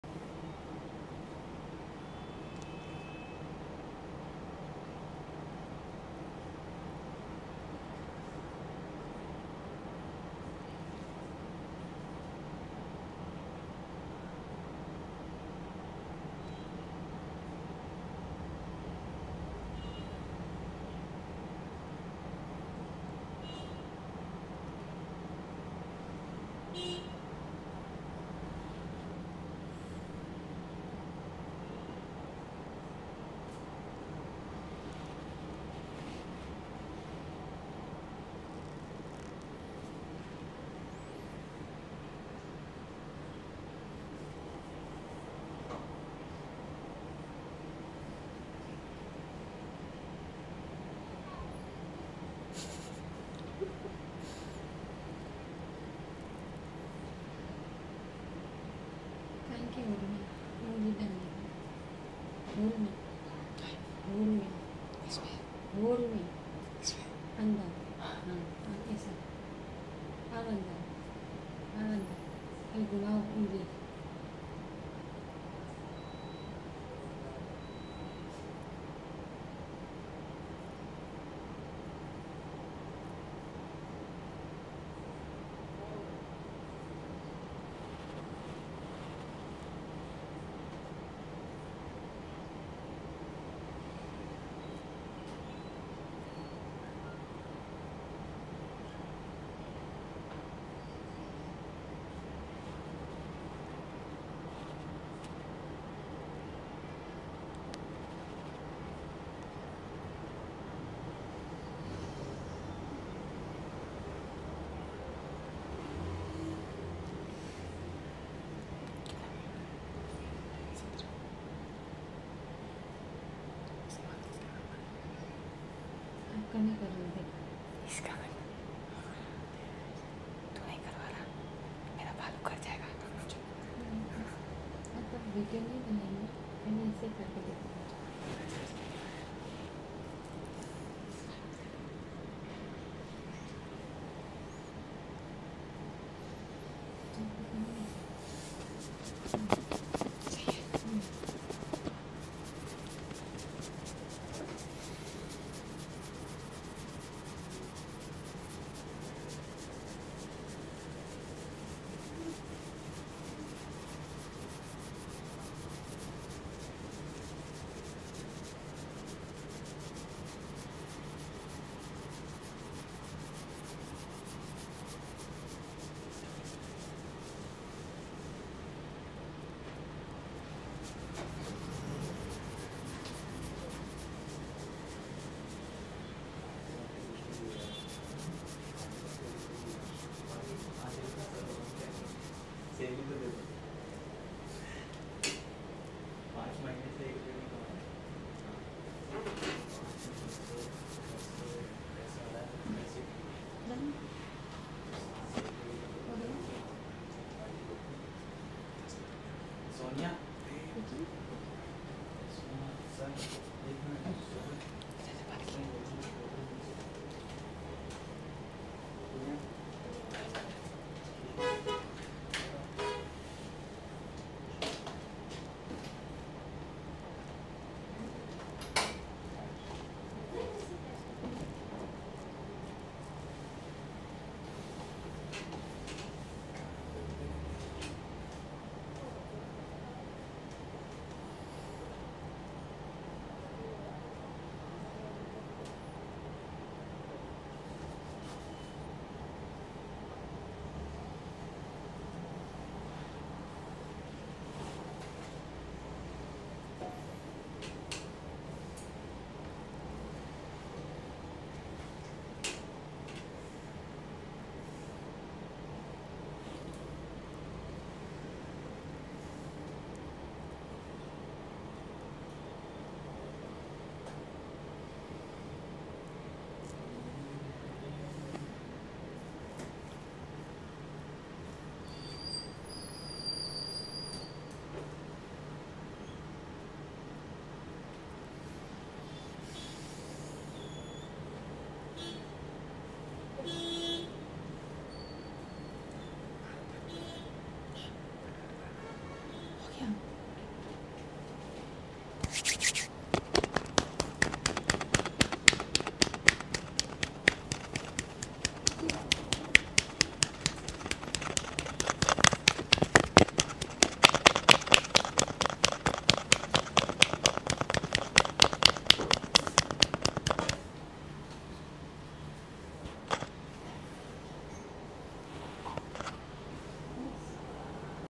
ASMR Deeply Relaxing Head & Scalp Oil Massage [ Therapy ] Relaxing Head Massage By Kabir | Part 4
Hi friends ,
We are together with a legendary video. This video will be our female physiotherapist technician friend. In the video there will be chair massage, back massage,Head massage, neck massage, arm massage, sleep massage. Thank you from now . Yours truly .